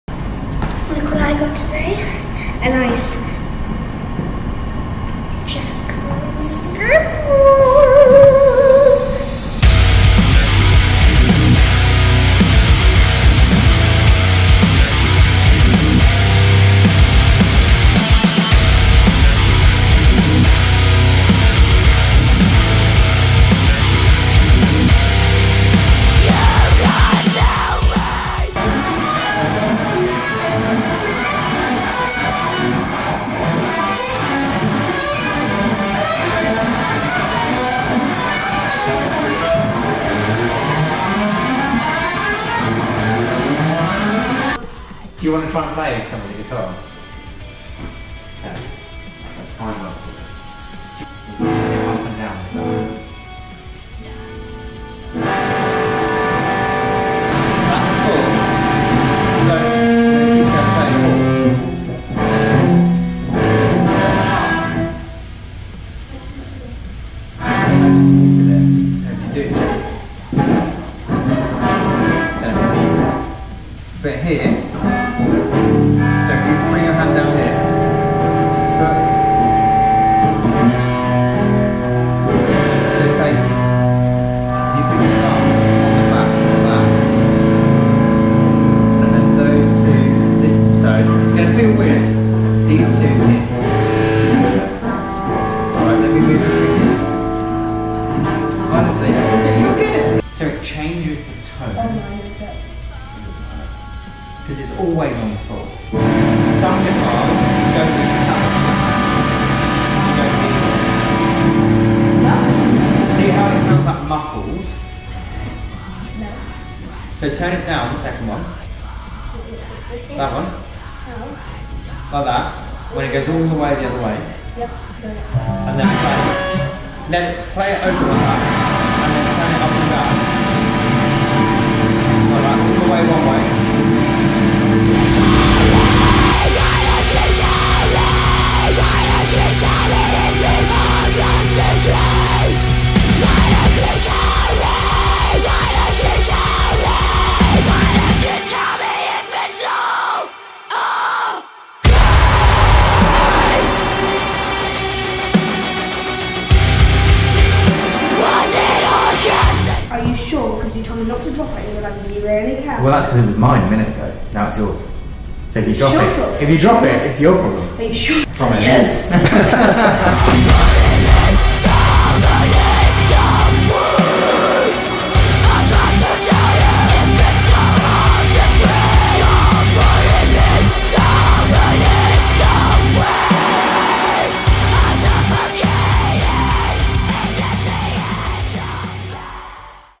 I went to Fender to get my first Jackson guitar!
I recently was lucky enough to visit Fender in London, I got shown around the shop and told a bunch of cool guitar history! Massive thank you to Tim Hillier-Brook for showing me around and giving me my first Jackson guitar!